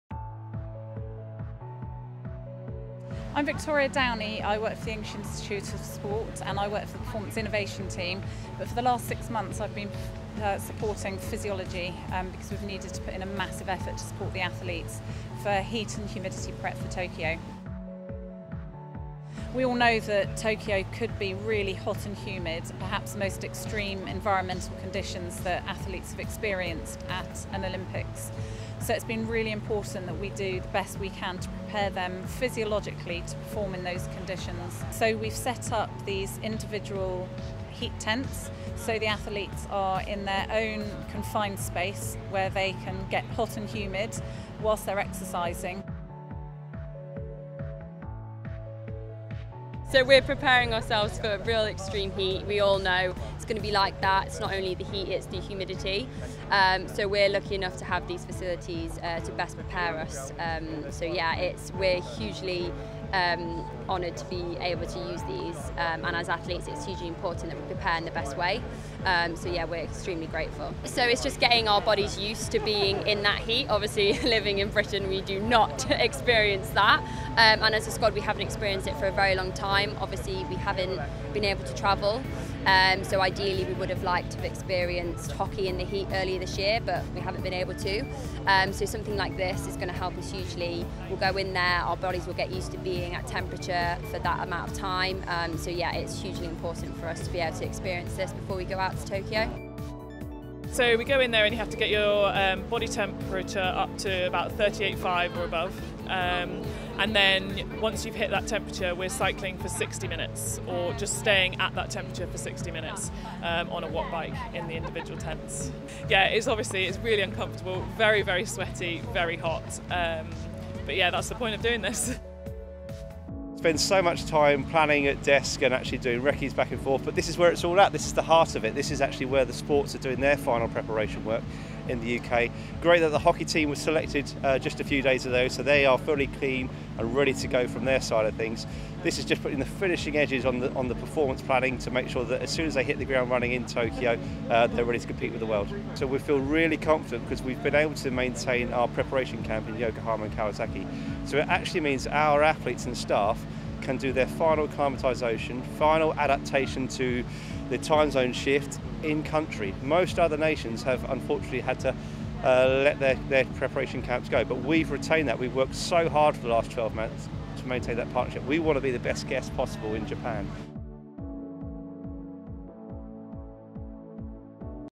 Team GB's hockey stars turn up the heat for their Tokyo preparations
The EIS, Team GB and GB Hockey have been preparing athletes for the temperatures they will face at the Olympic Games in Tokyo. Athletes at Bisham Abbey have been using individual, outdoor heat tents to acclimatise to the high temperatures while maintaining a Covid-safe environment.